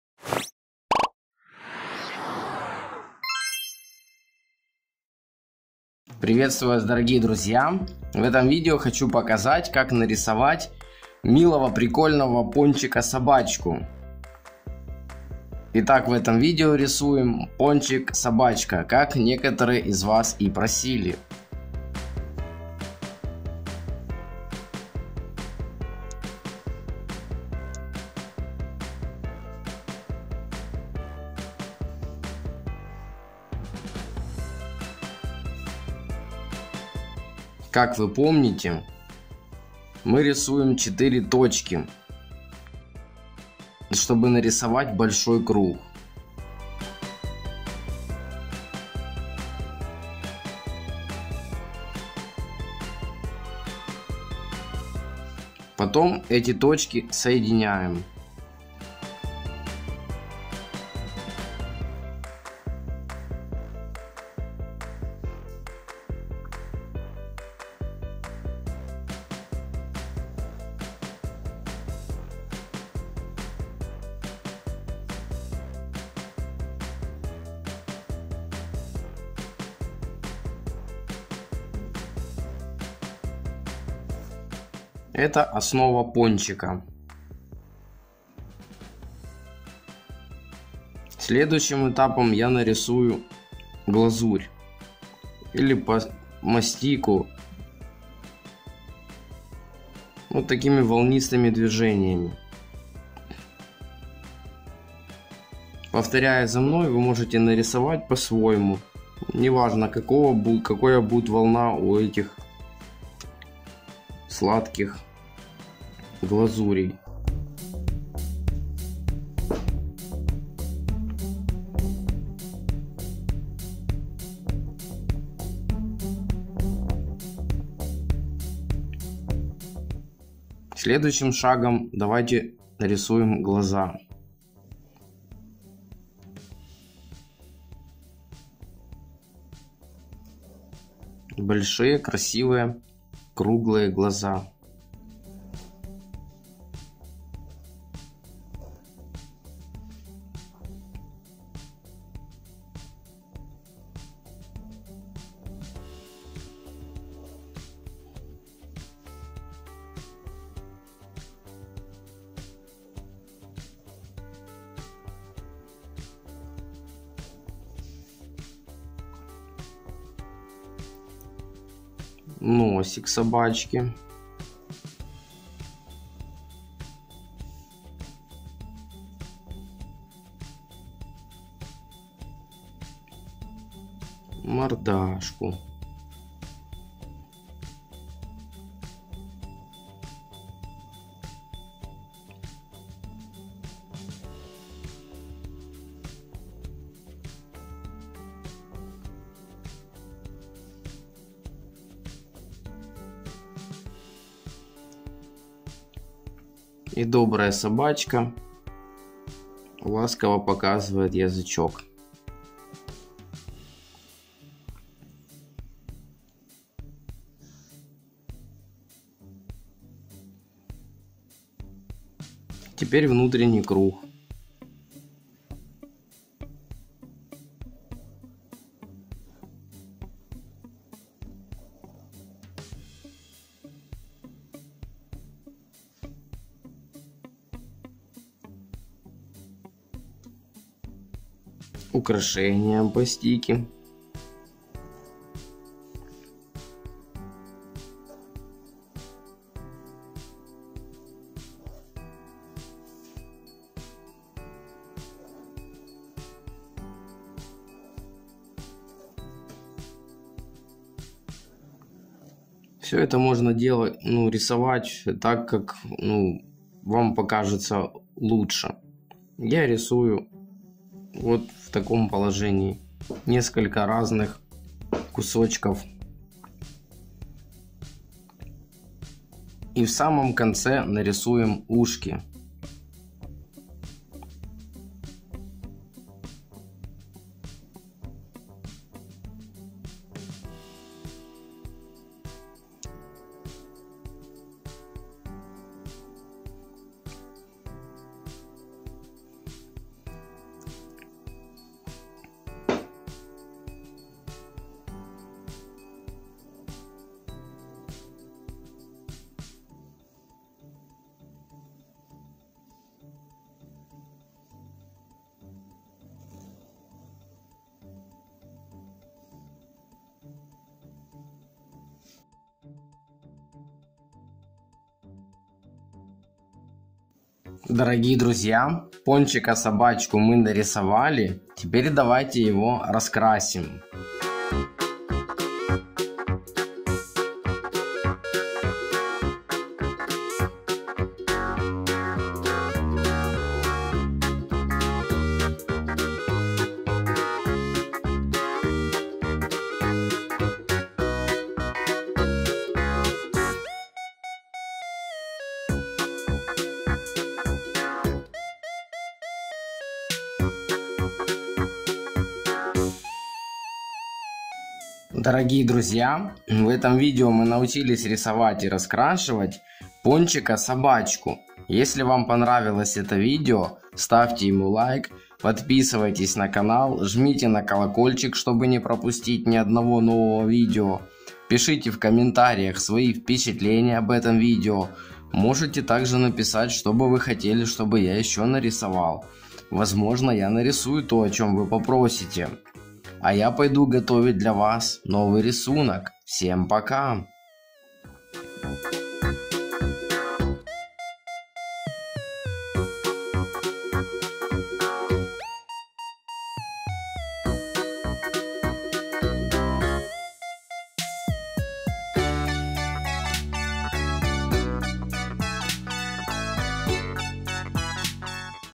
How To Draw A Cute Donut Dog | Drawing coloring
Hello! See in this video how to draw a cute donut dog. Hope you enjoy this fascinating special video. Love you!